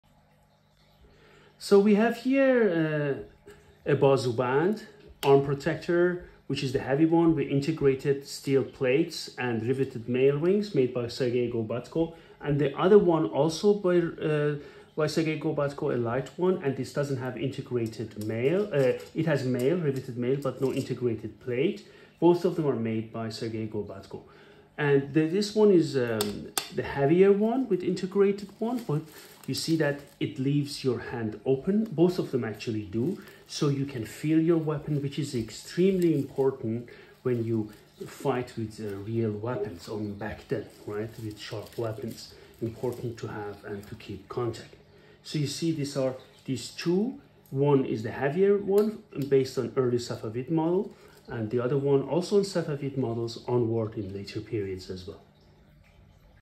Two different bazubands (armguards) made by Sergey Gorbatko
The following video shows two different armguards made by Sergey Grobatko: a heavy armguard with integrated steel plates and riveted mail rings for hand protection and b) ab armguard with riveted mail rings for hand protection. For the former example see the following video: